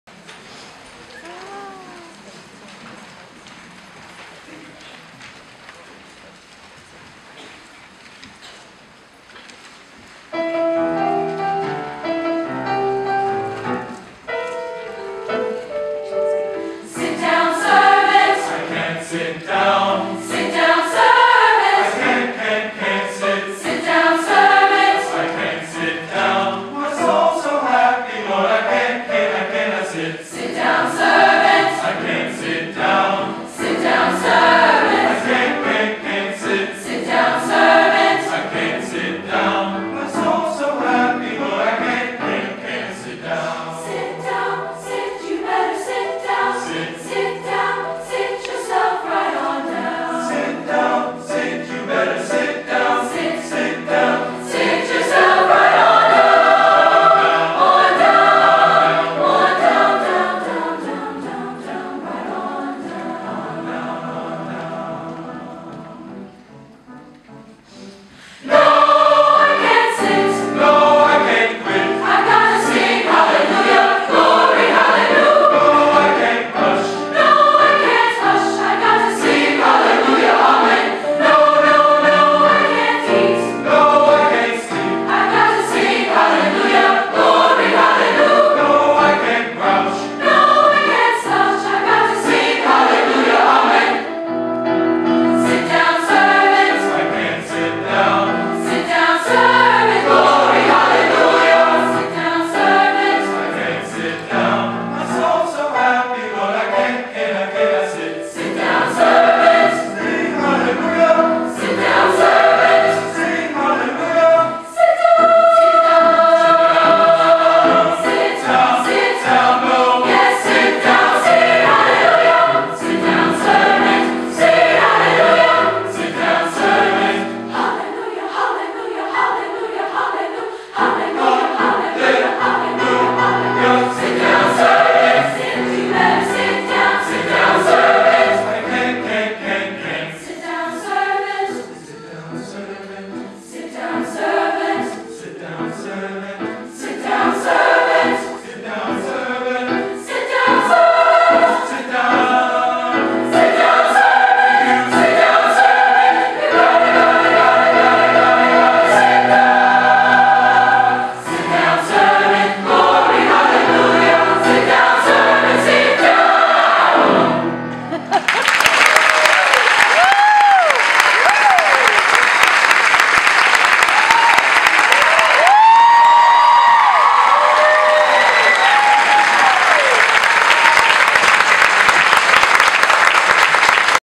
Cameron and his choir at U of Montevallo Music Camp, June 2017
My grandson, Cameron. and his friends at U of Montevallo Musicians Camp, June 2017.